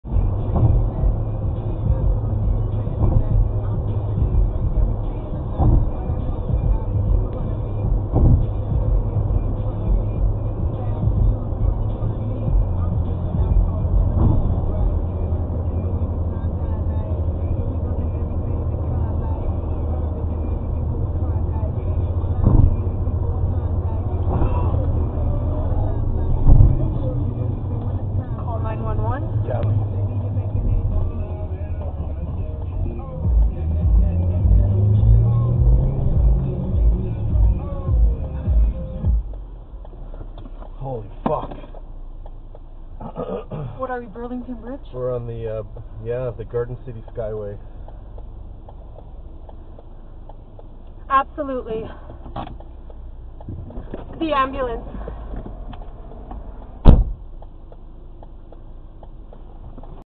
QEW Niagara Fort Erie-bound Garden City Skyway Accident
Accident recorded by dash cam